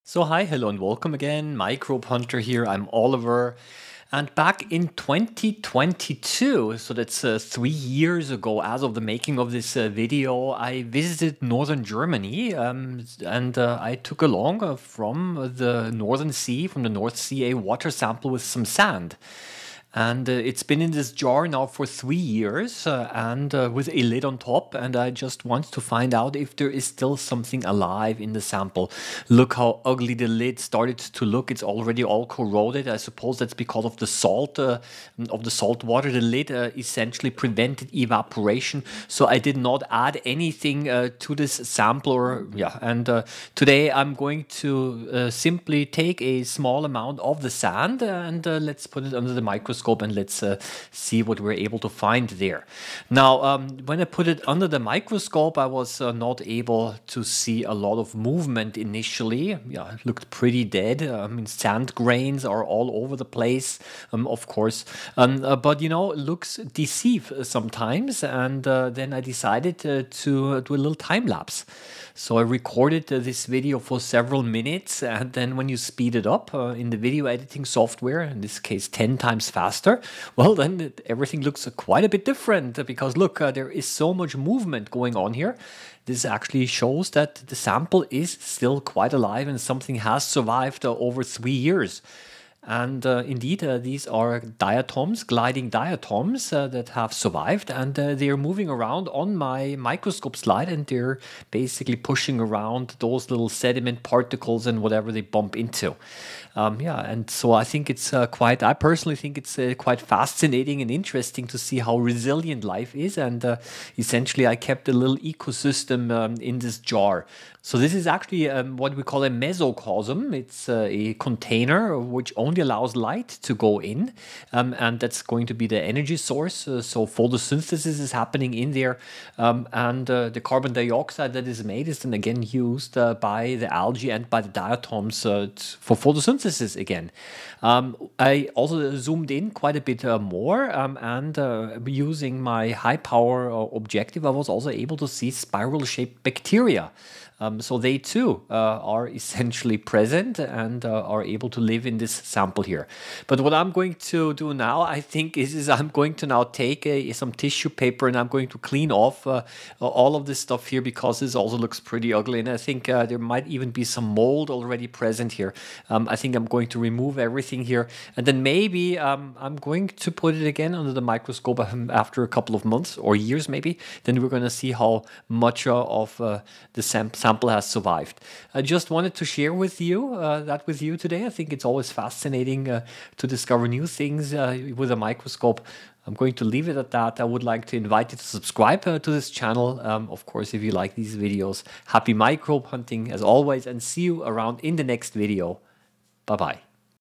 🔬 Can Microbes Survive 3 Years In This Jar? - Use TIMELAPSE to find out!
We have to do a bit of microscopy to find out if anything is still alive in this jar!